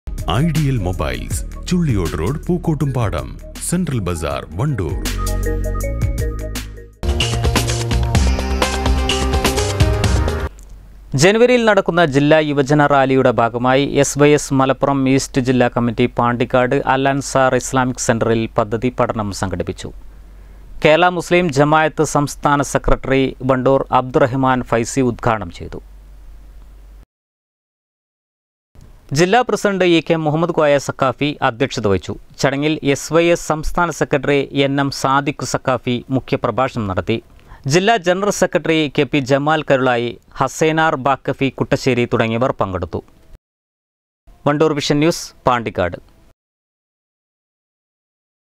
ജില്ലാ യുവജന റാലിയുടെ ഭാഗമായി SYS പദ്ധതി പഠനം സംഘടിപ്പിച്ചു.
wandoorvision news